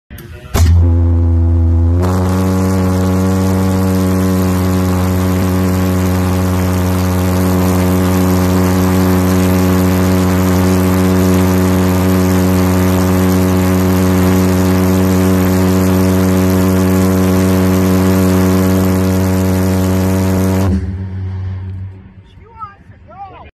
Snapper Comet Pulse Jet Mower Test start
Test start for new Snapper Comet lawnmower with 3 mounted pulse jets.